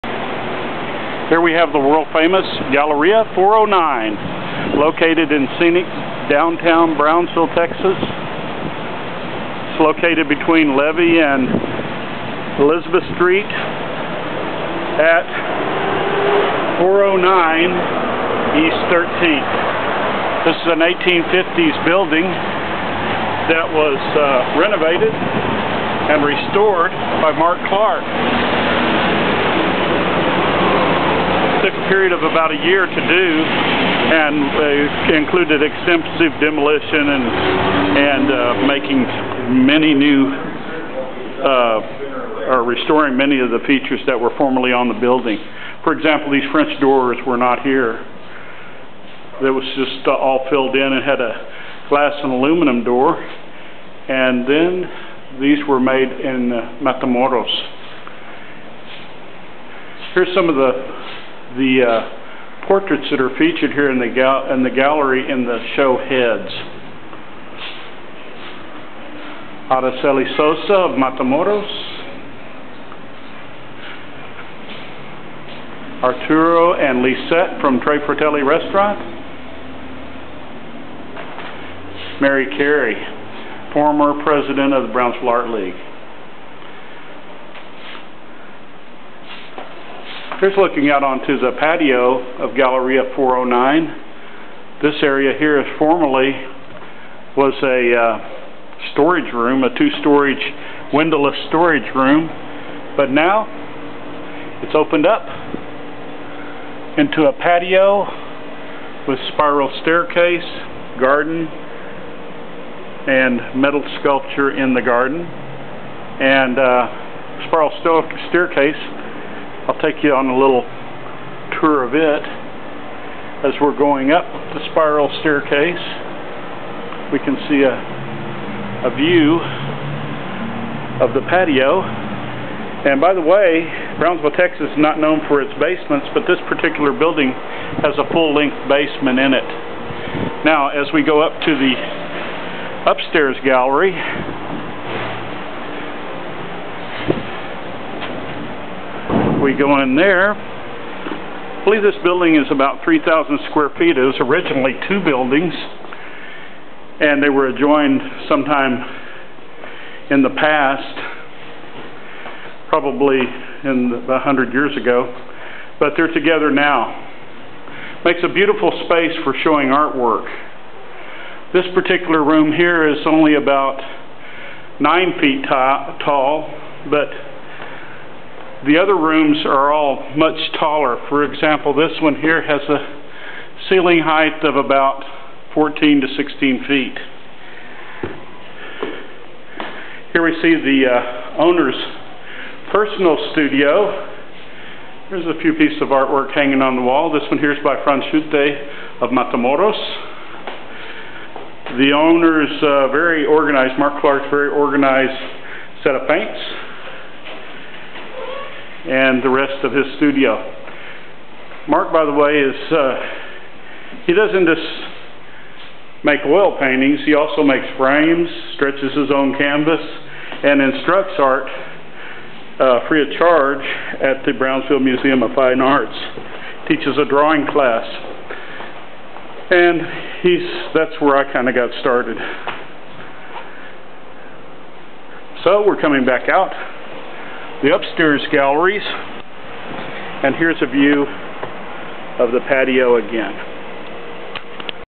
Art-Iguana takes a tour of Galeria' 409 , Brownsville, TX
Guided tour of Galeria' 409 and artist Cliff Welty's (alias--art-iguana) wild and crazy oil on linen portraits.